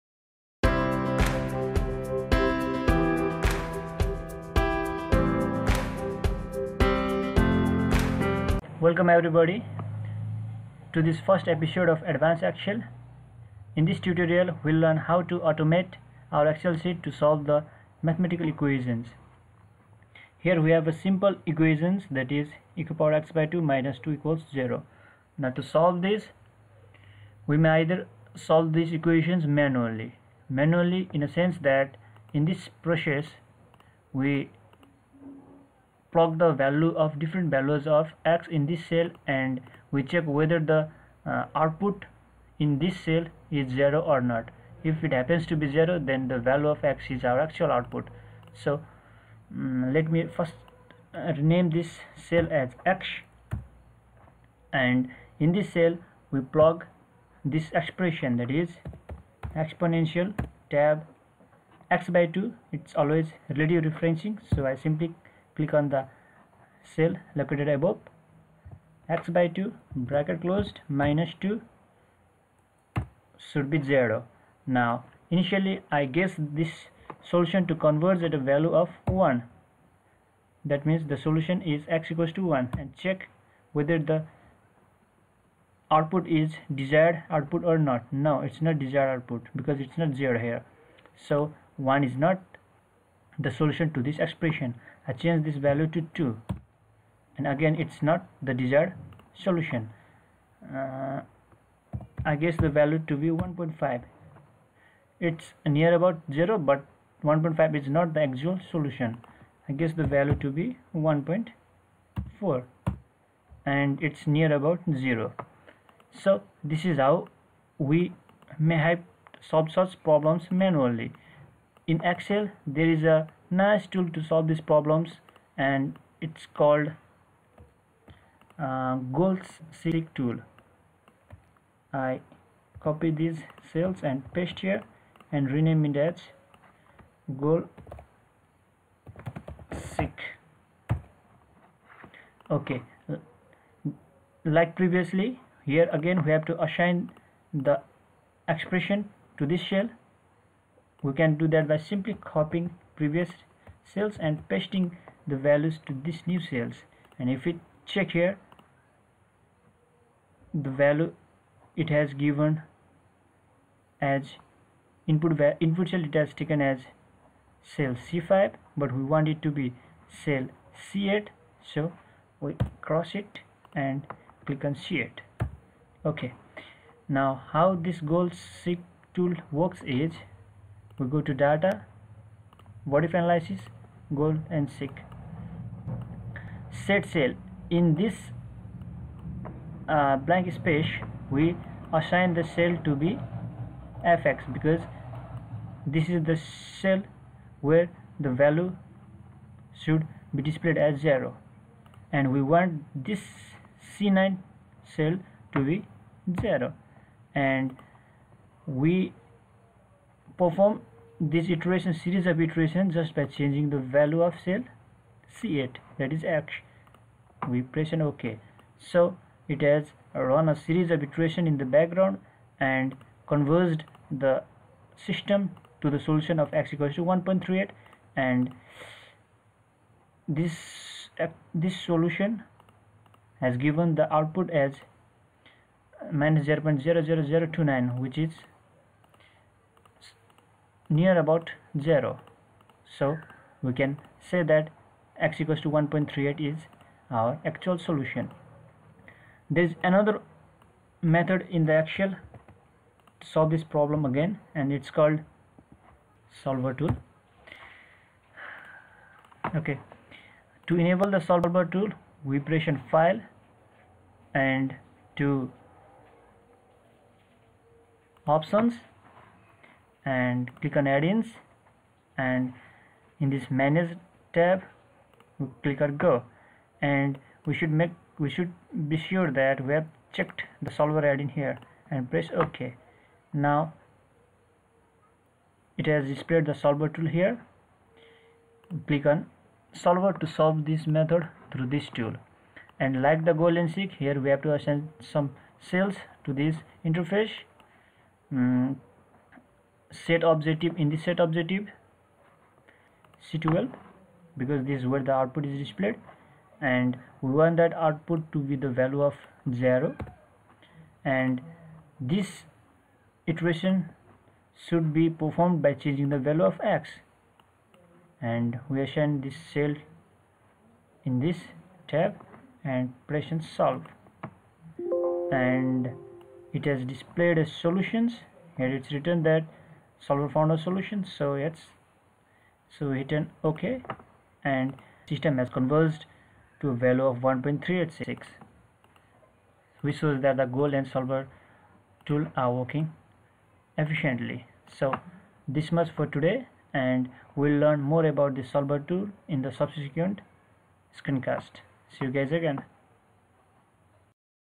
Excel sheet automation | Solving for x | Goal seek | Solver [Ep:01]
This is the first episode of the Advanced Excel-cum-VBA series. Here, you will learn how to solve an equation automatically using built-in tools like "Goal Seek" and "Solver" in Excel.